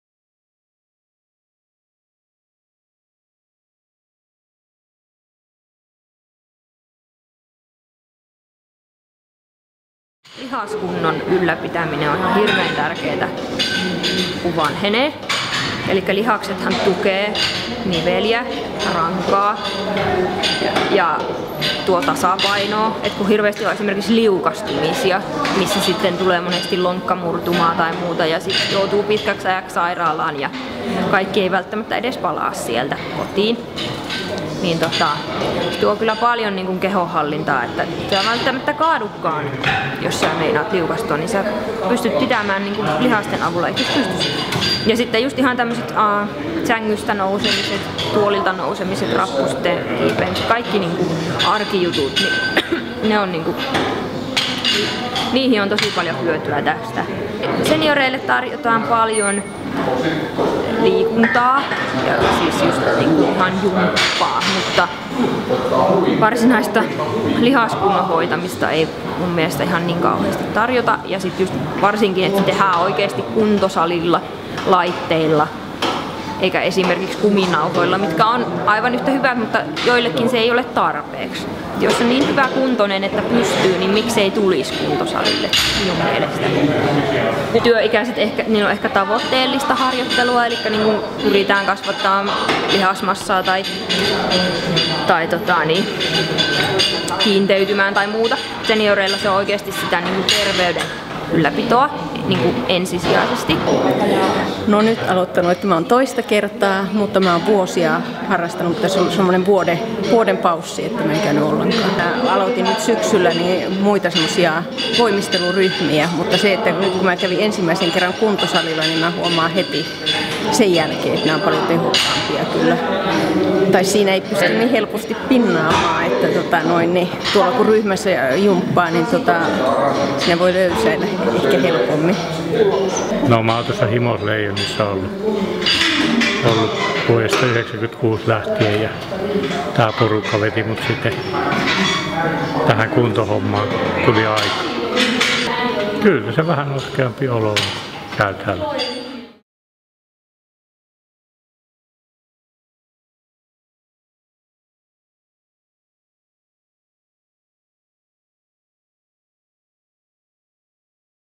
Senioreiden kuntosaliharjoittelu
Kynnys kuntosalilla käymiseen on madaltunut kaikilla ikäryhmillä, myös senioreilla.
Tämä näkyy muun muassa kuntosali Keskikehossa, jossa pari viikkoa sitten alkaneet senioreiden ohjatut kuntosaliryhmät ovat houkutelleet jo yli 140 eläkeikäistä liikkumaan.
Harva lähtee hankkimaan kuntosalijäsenyyttä yksin. Kavereiden houkuttelemana ja heidän kanssaan kuntoilu on mukavampaa.
Kuntosaliohjaaja ja kuntoilijat kertovat, miksi senioreiden kannattaa lähteä harjoittamaan lihaskuntoaan juuri salille.